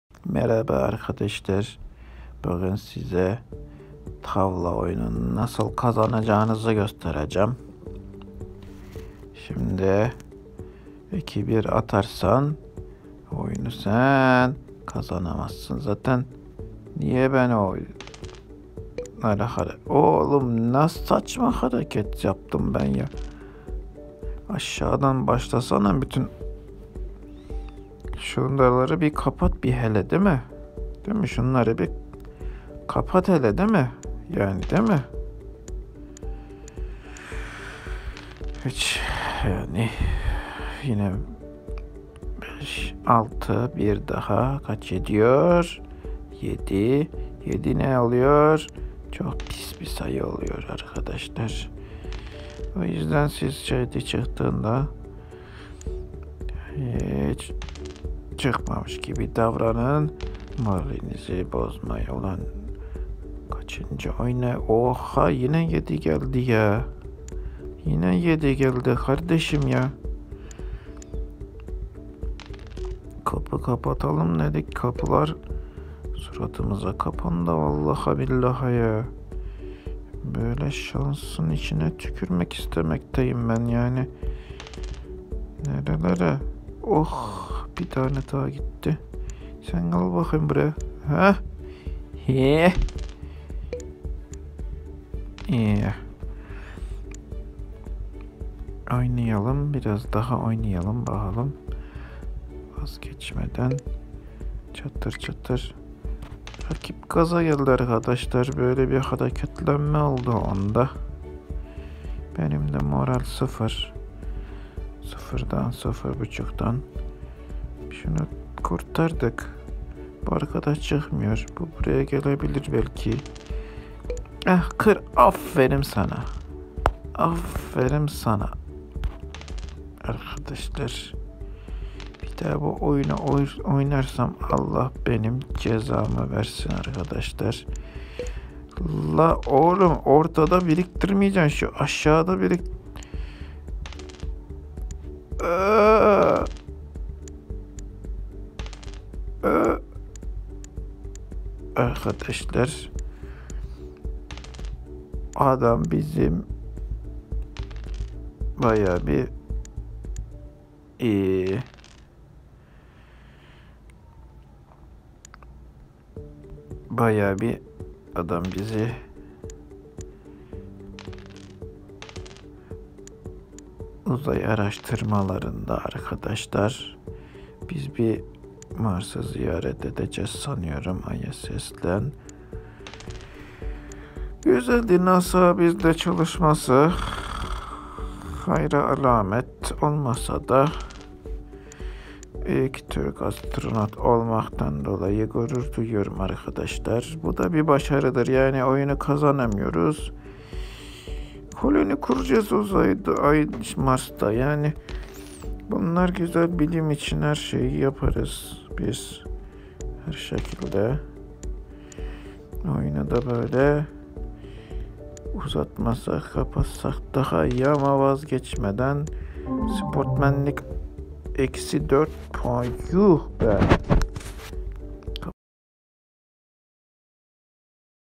Tavla Nasıl Oynanmaz (Tutorial)
Arkadaşlar bu videoda trajedi oyunu backgammon nam-ı diğer tavlanın nasıl oynanMAması gerektiğini anlattık ve uzay araştırmalarına katkıda bulunduk :S